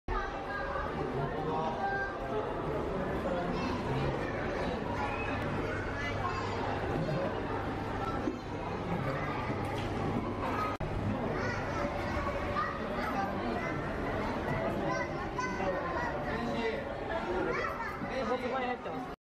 タコの隠れ蓑術！ 東京湾を再現した水槽（短い動画） Octopus, Tokyo Bay
葛西臨海水族園。
東京湾を再現した水槽です。
タコがいました。
/2017.9
： kasai＃27

いつもご視聴をして頂き、ありがとうございます。
kyuru kyuruでは、実際に撮り歩きをし、
１つ１つ動画を作って配信しております。
動画も高評価をいただけると嬉しいです。